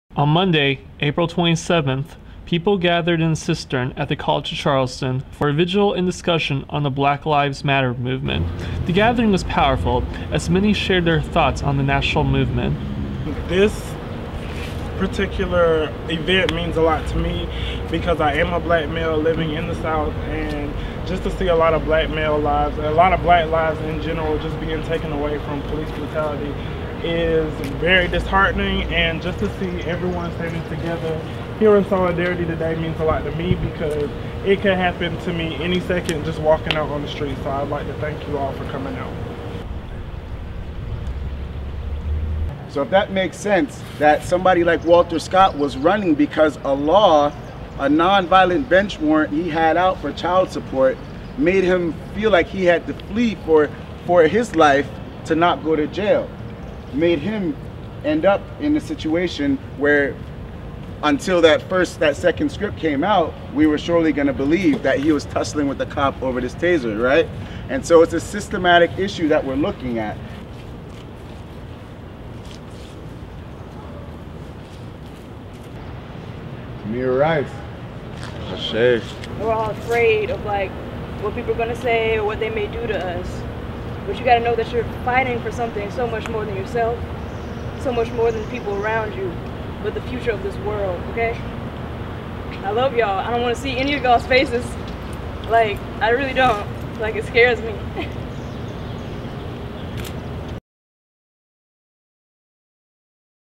BlackLivesMatter Gathering at the College of Charleston
On Monday, April 27th, people came together for the BlackLivesMatter movement in the cistern at the College of Charleston. It was powerful meeting, as many shared their thoughts and paid their respects to the recently deceased.